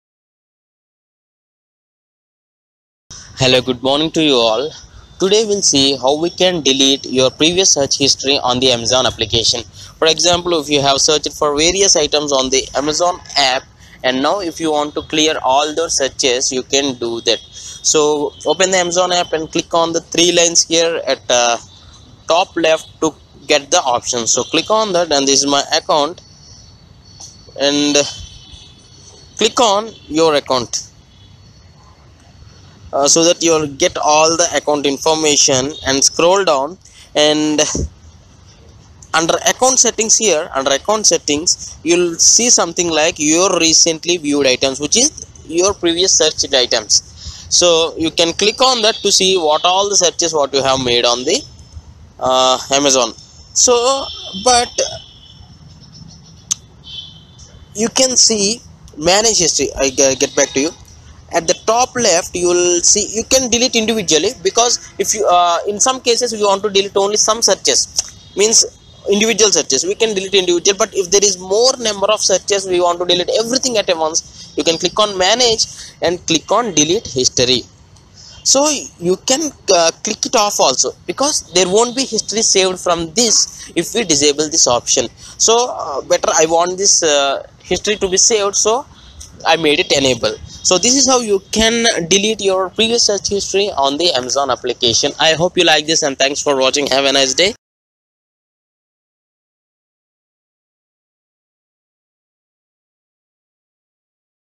How to Clear Search History Amazon
Clear all your previous search history on the amazon application what all you have searched for in the amazon,you can delete by each item or we can delete all the search at a time